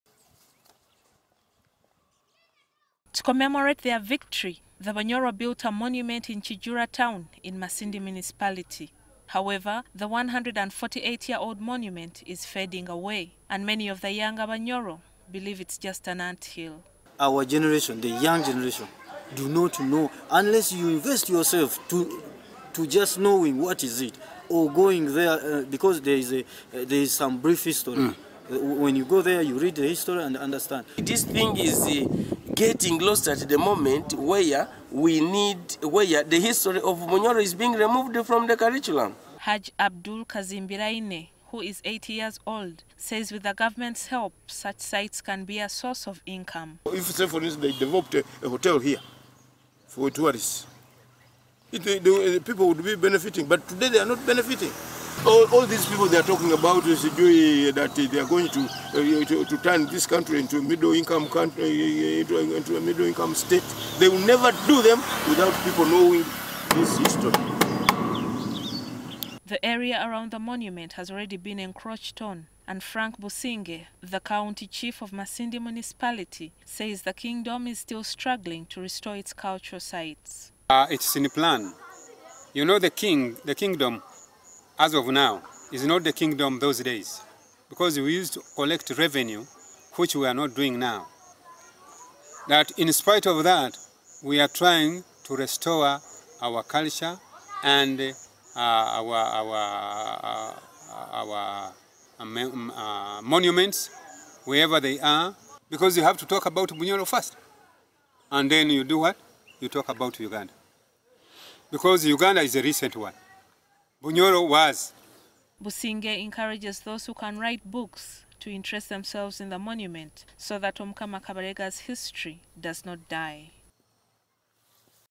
Retracing Kabalega’s iconic win over the British
The battlefield of Baligota Isansa is fading away. But not the memory attached to it. On 11th, 13th and 14th June, 1872 the armies of Omukama Kabalega of Bunyoro Kitara kingdom and British Mercenary Sir Samuel Baker fought a bitter battle in this place.Sir Baker needed more soldiers and his men started kidnapping Kabalega’s chiefs. Kabelega defeated Baker who retreated to Chobe inside what is now Murchison falls National park.